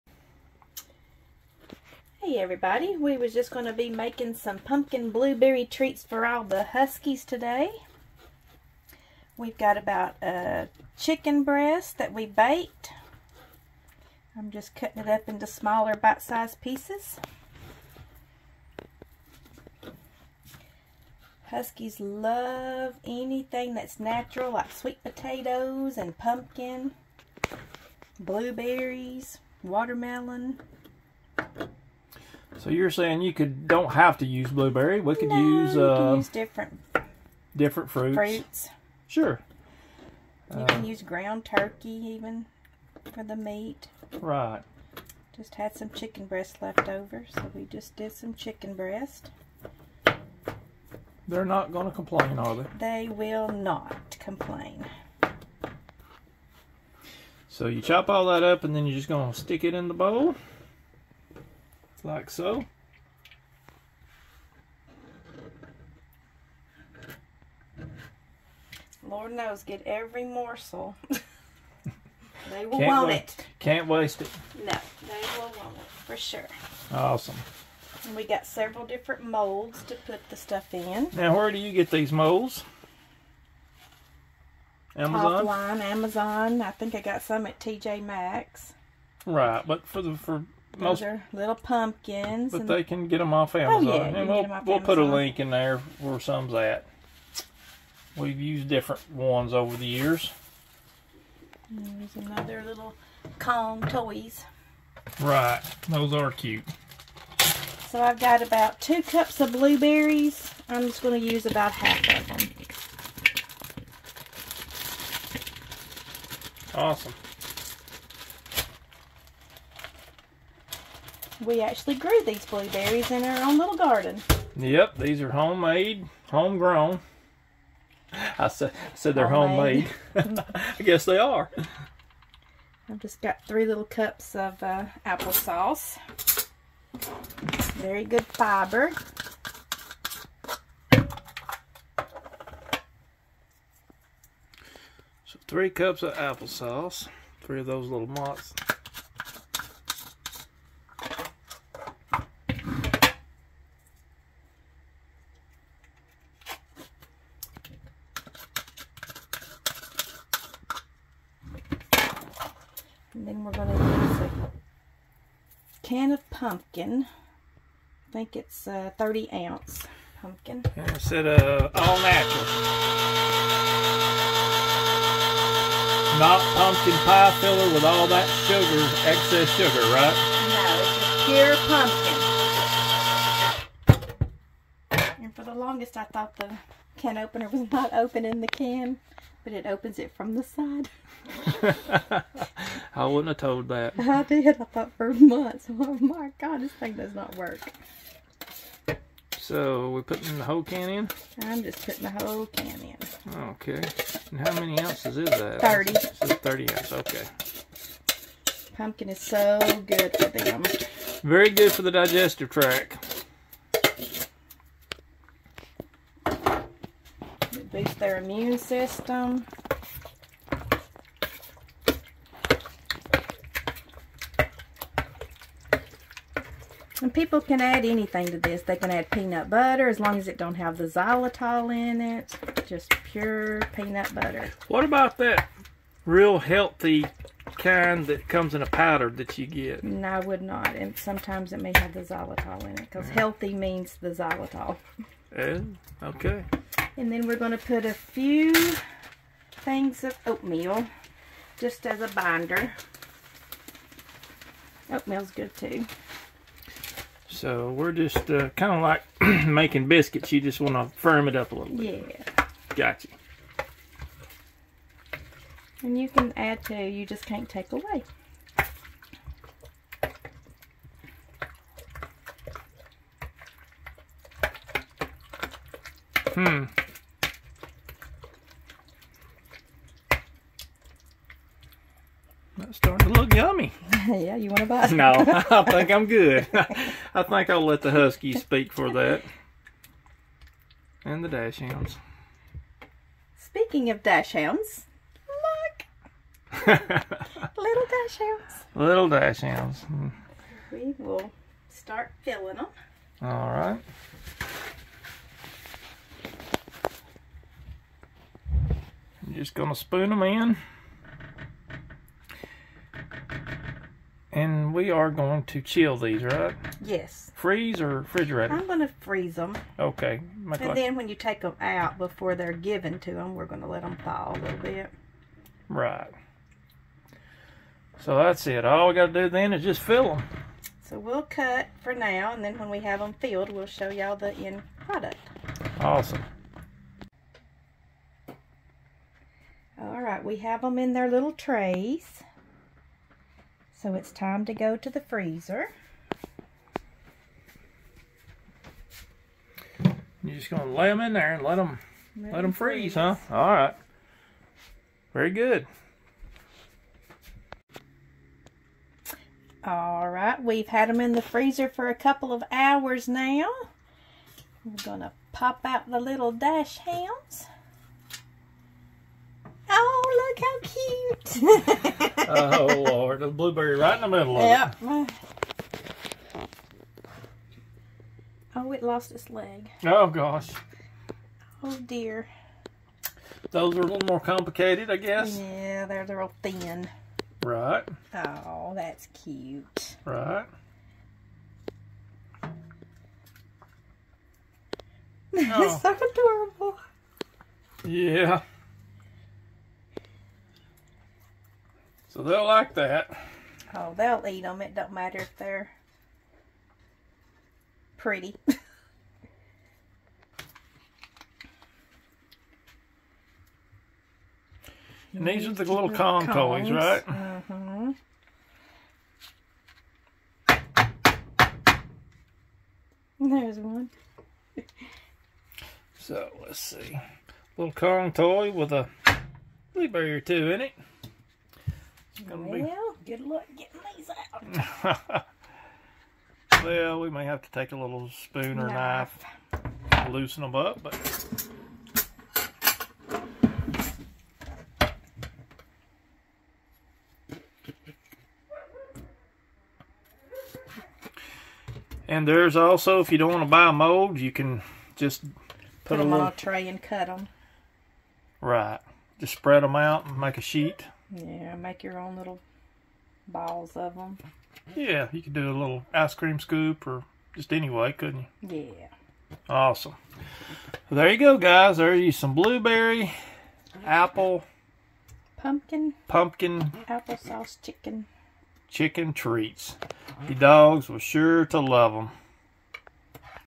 Homemade Chicken, Blueberry, & Pumpkin Dog Treats for Huskies!
In this video, we’re whipping up some delicious, healthy, and easy homemade dog treats for our huskies! 🐾 Using just three wholesome ingredients—chicken, blueberries, and pumpkin—we’ve created a recipe that’s not only packed with nutrients but also full of flavors dogs love.

Watch as our huskies eagerly wait for their special snacks while we walk you through the simple steps to make these treats at home. Perfect for pet parents looking to give their pups something extra special!

Why These Ingredients?
🍗 Chicken: High-protein goodness to fuel active dogs.
🫐 Blueberries: A superfood rich in antioxidants and vitamins.
🎃 Pumpkin: Great for digestion and adds a touch of natural sweetness.